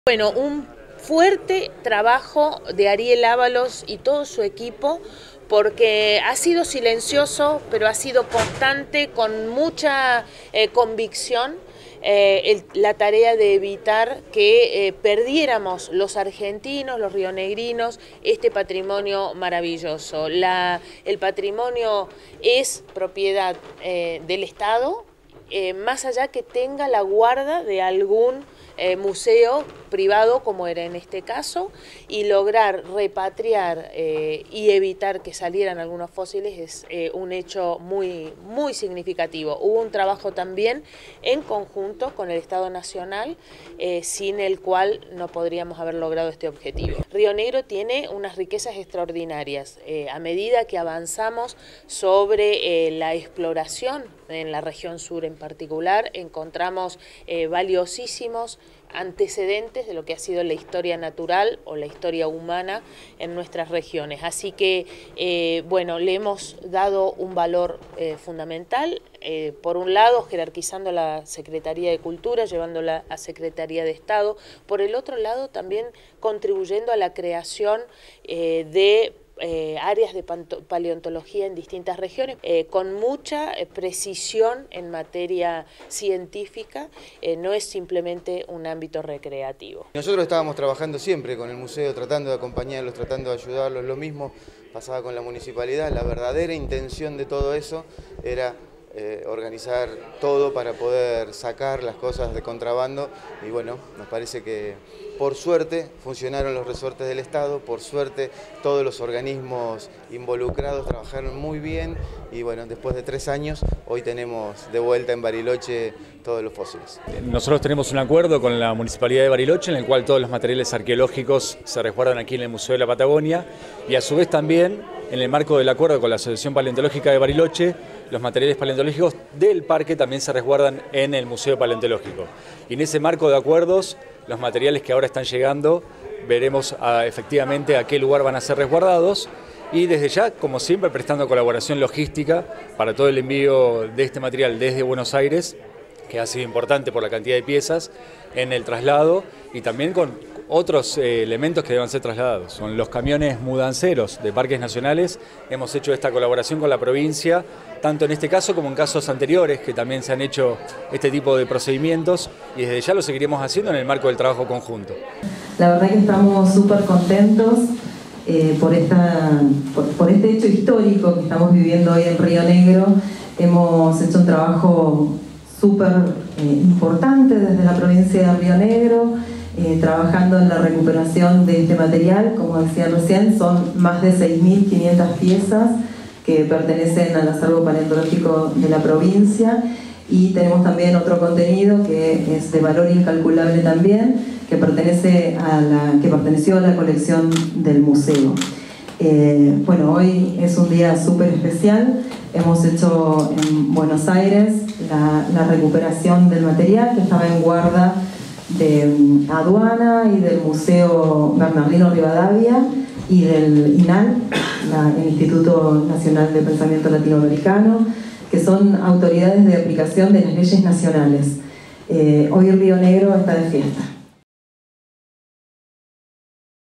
Histórico: más de 6400 fósiles recuperados volvieron a Bariloche
Arabela Carreras, gobernadora de Rio Negro
Ariel Avalos, secretario de Estado de Cultura
Horacio Paradela, intendente Parque Nacional Nahuel Huapi
Natalia Villegas, subsecretaria de Patrimonio y Cooperación Cultural provincial